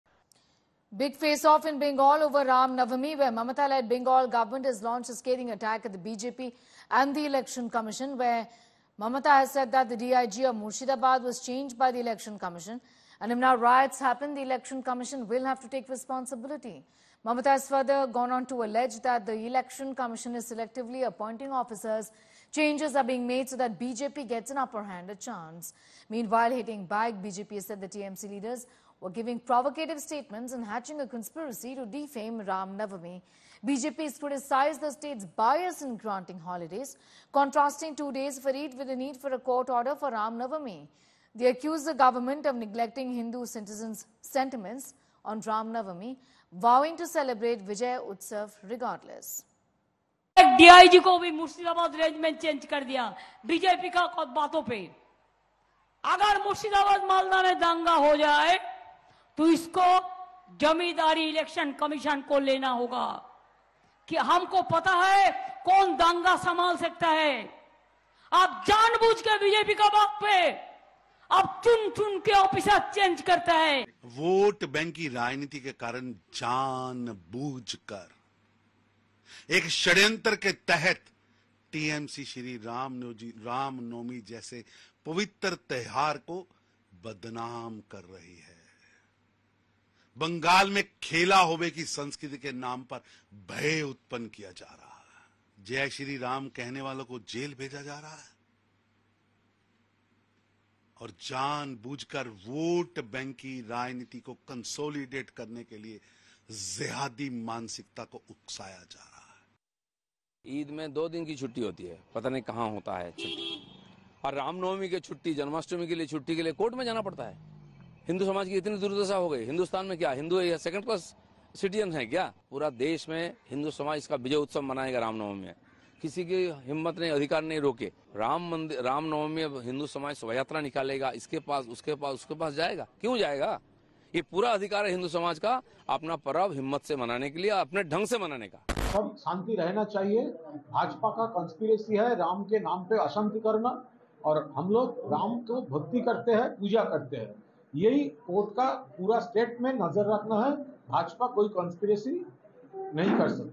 'TMC Hurts Hindu Sentiments' BJP-TMC's Mega Faceoff In West Bengal Over Ram Navami | India Today
Prime Minister Narendra Modi, addressing a poll rally in West Bengal’s Balurghat, alleged that the Trinamool Congress tried to stop Ram Navami celebrations while the Ram Temple inauguration was happening.

“At a time when Ram Lalla has taken his place at the magnificent Ram Temple in Ayodhya, I was told the TMC has done everything to halt the Ram Navami celebration in Bengal. But truth always prevails, and so the court has given permission for massive celebrations,” PM Modi said.